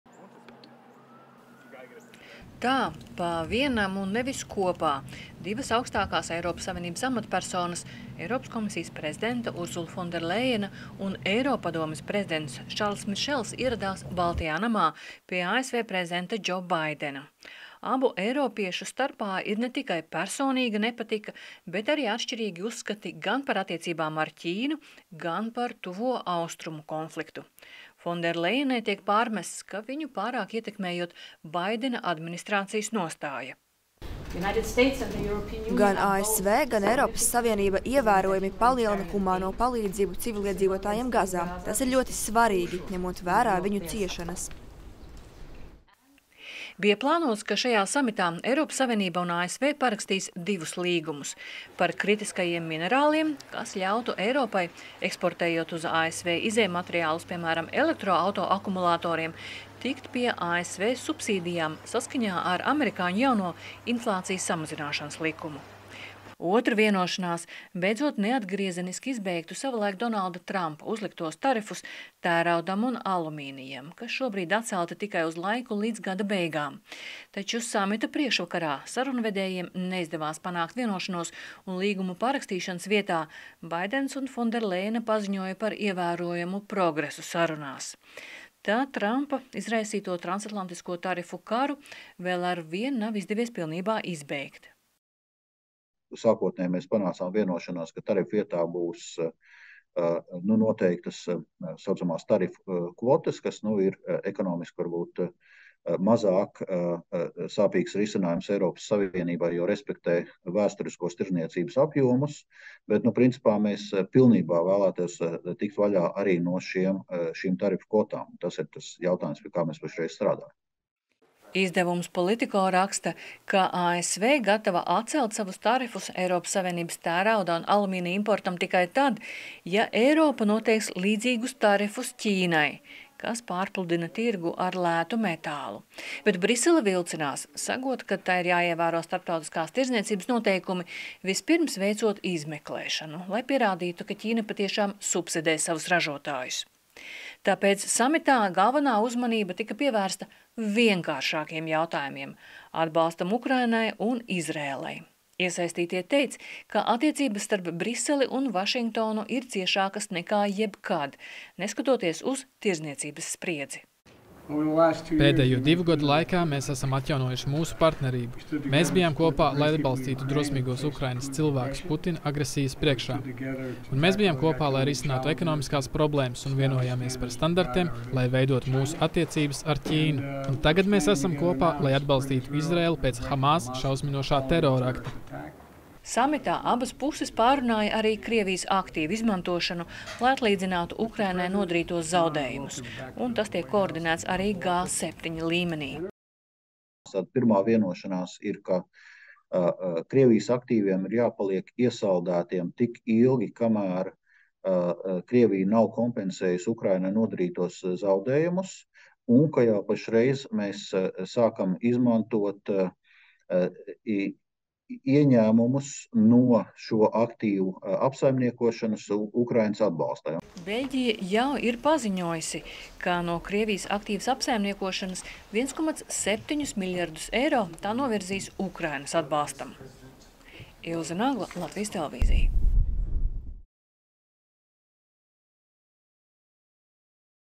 ES – ASV samits vēl neizbeidz transatlantisko tarifu karu
Vašingtonā noslēdzies pirmais ES un ASV samits vairāk nekā divu gadu laikā. Tā uzmanības fokusā vajadzēja būt ieilgušās spriedzes kliedēšanai tirdzniecības attiecībās starp abiem sabiedrotajiem. To līdz galam neizdevās panākt un šo strīdu aizēnoja arī notikumi Tuvajos Austrumos.